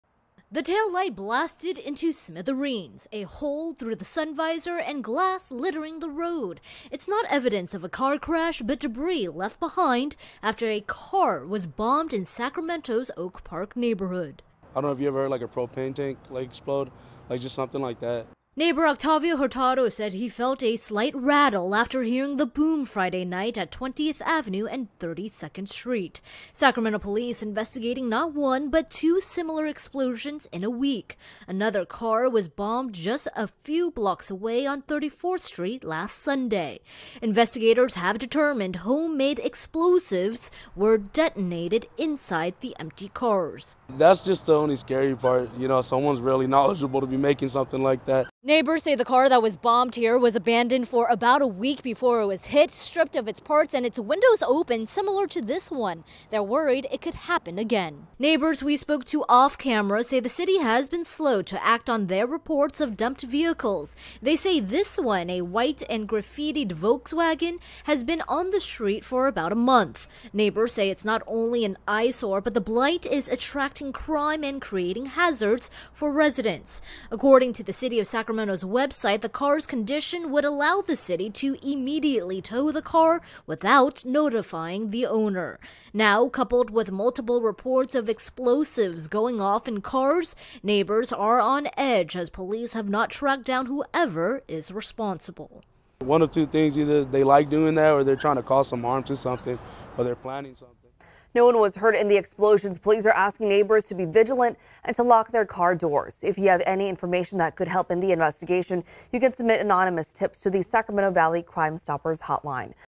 Car explosions under investigation in Sacramento neighborhood
The Sacramento Police Department has launched an investigation after there were car explosions in an Oak Park neighborhood. No one was hurt in the explosions.